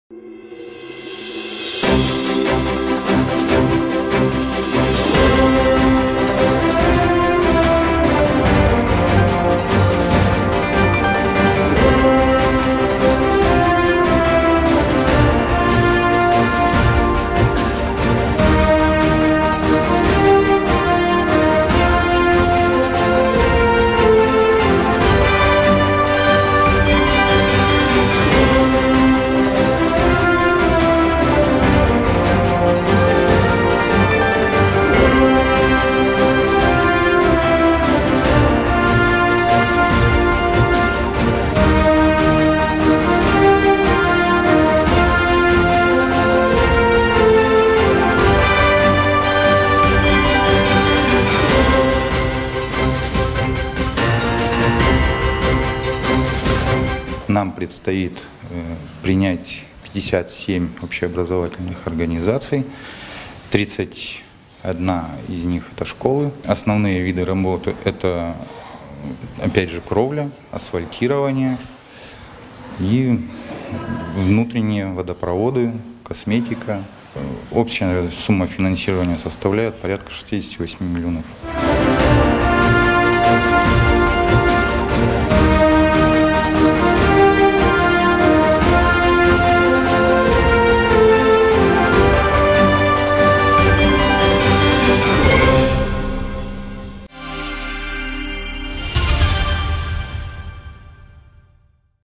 В Вологде завершаются проверки образовательных учреждений к новому учебному году
Детские сады и школы проверяет межведомственная комиссия. В нее входят сотрудники Роспотребнадзора, МЧС, полиции и управления образования. Смотрят, как выполнены ремонты, насколько помещения соответствуют всем нормам. Так, в детском саду № 67 на ул. Пугачева, 7, этим летом прошел комплексный ремонт. Отремонтировали кровлю, класс для музыкальных и физкультурных занятий,установили новую рабицу и покрасили веранды.  На замену кровли из бюджета города было выделено более 780 тыс. рублей. Приемка пройдет во всех образовательных учреждениях города до 7 августа. Напомним, проверки учреждений образования начались 3 июля. На первом этапе было проверено 74 учреждения